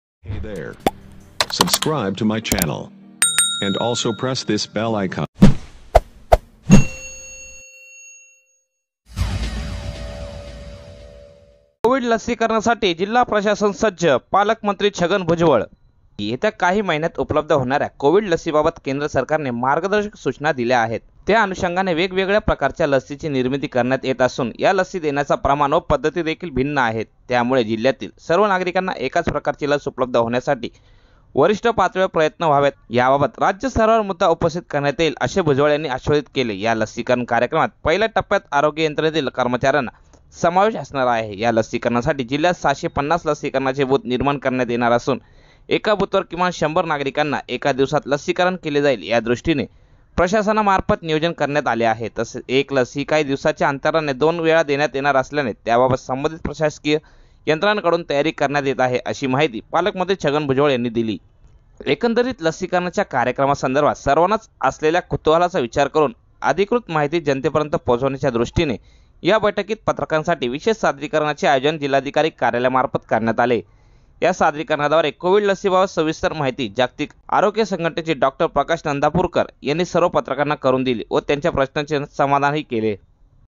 Daksh News Live | कोविड लसीकरणासाठी जिल्हा प्रशासन सज्ज: पालकमंत्री छगन भुजबळ
*कोविड लसीकरणासाठी जिल्हा प्रशासन सज्ज*
               *: पालकमंत्री छगन भुजबळ*

 *शहरासह ग्रामीण भागातील 9 वी ते 12 वीचे वर्ग होणार सुरू*

नाशिक, दि. 19 डिसेंबर 2020 (जिमाका वृत्तसेवा):
जिल्ह्यातील ग्रामीण व शहरी भागातील इयत्ता 9 वी ते 12 वी पर्यंतच्या शाळा 4 जानेवारी पासून सुरू करण्यात येणार असून त्यादृष्टिने जिल्हा परिषद, महानगरपालिका व आरोग्य या प्रशासकीय यंत्रणांनी नियोजन करावे. तसेच कोविड लसीकरणाचा कार्यक्रम संपूर्ण जिल्ह्यात राबविण्यासाठी जिल्हा प्रशासनामार्फत पूर्व तयारी करण्यात आली असून प्रशासकीय यंत्रणा लसीकरणासाठी सज्ज आहेत, असे प्रतिपादन राज्याचे अन्न, नागरी पुरवठा व ग्राहक संरक्षणमंत्री तथा जिल्ह्याचे पालकमंत्री छागन भुजबळ यांनी केले आहे.

जिल्हाधिकारी कार्यालयाच्या मध्यवर्ती सभागृहात आयोजित करण्यात आलेल्या कोरोना आढावा बैठकीत पालकमंत्री श्री. भुजबळ बोलत होते. यावेळी जिल्हाधिकारी सूरज मांढरे, पोलिस आयुक्त दिपक पाण्डेय्, महानगरपालिका आयुक्त कैलास जाधव, जिल्हा परिषदेच्या मुख्य कार्यकारी अधिकारी लीना बनसोड, जिल्हा पोलिस अधिक्षक सचिन पाटील, मालेगाव महानगरपालिका आयूक्त दिपक कासार, जिल्हा शल्यचिकित्सक डॉ. रत्ना रावखंडे, उपजिल्हाधिकारी वासंती माळी, जिल्हा आरोग्य अधिकारी डॉ. कपिल आहेर, महानगरपालिकेचे आरोग्य अधिकारी डॉ. बापूसाहेब नागरगोजे, महानगरपालिका नोडल अधिकारी डॉ. आवेश पल्लोड, आगतिक आरोग्य संघटनेचे डॉ. प्रकाश नंदापूरकर, डॉ. उत्कर्ष दुधडीया आदी उपस्थित होते.

यावेळी पालकमंत्री श्री. भुजबळ म्हणाले, जिल्ह्यात 4 जानेवारीपासून शाळा सुरू करण्याच्या उद्देशाने महानगरपालिका, नगरपरिषदा तसेच आरोग्य यंत्रणेने शिक्षक व शिक्षकेतर कर्मचारी यांच्या कोविड चाचण्या करण्यासाठी प्रत्येक तालुक्यात सुक्ष्म नियोजन करण्यात यावे. जेणेकरून शाळा सुरू होण्याच्या कालावधीपर्यंत सर्व संबंधित शिक्षक व शाळेतील कर्मचारी वर्ग यांचा कोविड तपासणीचा रिपोर्ट वेळेत प्राप्त होवून शाळेत जाण्यासाठी शिक्षकांना अडचण निर्माण होणार नाही. त्याचप्रमाणे येत्या काही दिवसात सर्व शाळांचे निर्जंतुकीकरण करण्यात यावे. तसेच शाळा सुरू झाल्यानंतर शाळेत उपस्थित राहणाऱ्या प्रत्येकाला मास्कचा वापर, सुरक्षित अंतर व नियमित सॅनिटायझरचा वापर करणे अनिवार्य राहिल, असेही पालकमंत्री श्री. भुजबळ यांनी  सांगितले आहे.

गेल्या काही दिवसांपासून उपचार घेत असलेल्या रुग्णांची संख्या सातत्याने कमी होत आहे. तसेच राज्याच्या 2.6 टक्के मृत्यूदराच्या तुलनेत जिल्ह्याच्या मृत्यूदर 1.6 टक्के इतकाच असल्याने जिल्ह्यात कोरोनाच्या दुसऱ्या लाट येण्याची शक्यता कमी वाटते असे श्री भुजबळ म्हणाले.

येत्या काही महिन्यात उपलब्ध होणाऱ्या कोविड लसीबाबत केंद्र सरकारने मार्गदर्शक सुचना दिल्या आहेत. त्याअनुषंगाने वेगवेगळ्या प्रकारच्या लसींची निर्मीती करण्यात येत असून या लसी देण्याचे प्रमाण व पद्धती देखील भिन्न आहेत. त्यामुळे जिल्ह्यातील सर्व नागरिकांना एकाच प्रकारची लस उपलब्ध होण्यासाठी वरिष्ठ पातळीवरून प्रयत्न व्हावेत असे प्रतिपादन जिल्हाधिकारी श्री सुरज मांढरे यांनी बैठकीत केले. याबाबत राज्य स्तरावर मुद्दा उपस्थित करण्यात येईल असे भुजबळ यांनी आश्वासित केले.  या लसीकरण कार्यक्रमाच्या पहिल्या टप्प्यात आरोग्य यंत्रणेतील कर्मचाऱ्यांचा समावेश असणार आहे. या लसीकरणासाठी जिल्ह्यात 650 लसीकरणाचे बुथ निर्माण करण्यात येणार असून एका बुथवर किमान 100 नागरिकांना एका दिवसात लसीकरण केले जाईल, या दृष्टिने प्रशासनामार्फत नियोजन करण्यात आले आहे. तसेच एक लस ही काही दिवसांच्या अंतराने दोन वेळा देण्यात येणार असल्याने त्याबाबत संबंधित प्रशासकीय यंत्रणाकडून तयारी करण्यात येत आहे, अशी माहितीही पालकमंत्री  श्री. भुजबळ यांनी दिली आहे.

एकंदरीतच लसीकरणाच्या कार्यक्रमासंदर्भात सर्वांनाच असलेल्या कुतूहलाचा विचार करून अधिकृत माहिती जनतेपर्यंत पोहोचवण्याचे दृष्टीने या  बैठकीत पत्रकारांसाठी विशेष सादरीकरण याचे आयोजन जिल्हाधिकारी कार्यालयामार्फत करण्यात आले होते. या सादरीकरणाद्वारे कोविड लसीबाबतची सविस्तर माहिती जागतिक आरोग्य संघटनेचे डॉ. प्रकाश नंदापूरकर यांनी सर्व पत्रकारांना करून दिली व त्यांच्या प्रश्नांचे समाधानी केले.